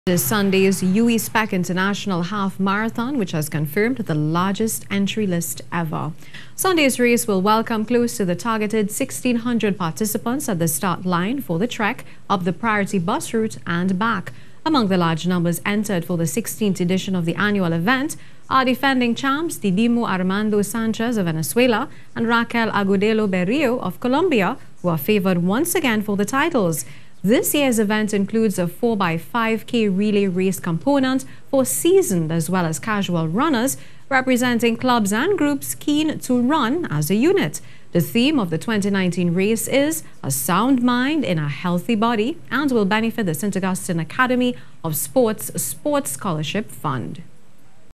SPORT: Record Figures For UWI/Spec Half Marathon
Sunday's UWI/SPEC International Half-Marathon will welcome close to the targeted 1,600 participants at the start line for the trek up the Priority Bus Route and back.